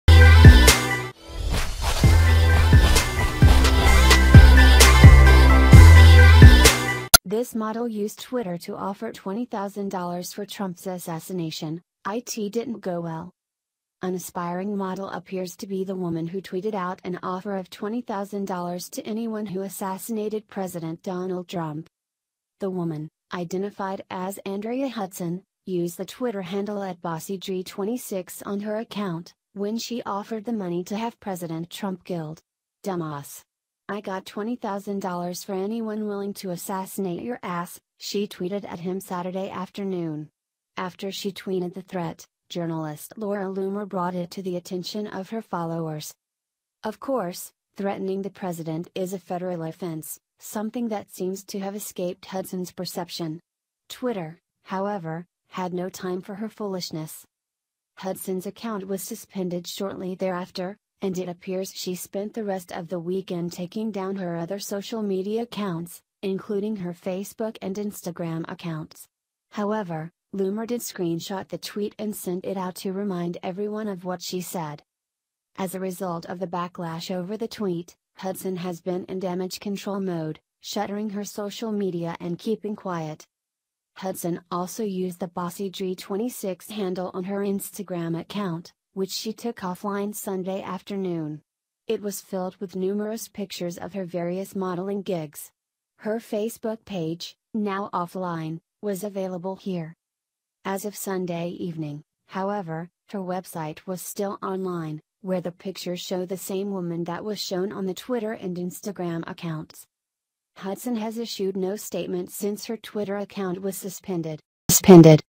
THIS MODEL USED TWITTER TO OFFER $20K FOR TRUMP’S ASSASSINATION – IT DIDN’T GO WELL
An aspiring model appears to be the woman who tweeted out an offer of $20,000 to anyone who assassinated President Donald Trump.
The woman, identified as Andrea Hudson, used the Twitter handle “@Bossydrea26” on her account, when she offered the money to have President Trump killed. “Dumbass. I got $20,000 for anyone willing to assassinate your ass,” she tweeted at him Saturday afternoon. After she tweeted the threat, journalist Laura Loomer brought it to the attention of her followers.
Of course, threatening the President is a federal offense, something that seems to have escaped Hudson’s perception. Twitter, however, had no time for her foolishness.

Hudson’s account was suspended shortly thereafter, and it appears she spent the rest of the weekend taking down her other social media accounts, including her Facebook and Instagram accounts. However, Loomer did screenshot the tweet and sent it out to remind everyone of what she said.

As a result of the backlash over the tweet, Hudson has been in damage-control mode, shuttering her social media and keeping quiet.

Hudson also used the “Bossydrea26” handle on her Instagram account, which she took offline Sunday afternoon. It was filled with numerous pictures of her various modeling gigs. Her Facebook page, now offline, was available here.
As of Sunday evening, however, her website was still online, where the pictures show the same woman that was shown on the Twitter and Instagram accounts.

Hudson has issued no statement since her Twitter account was suspended.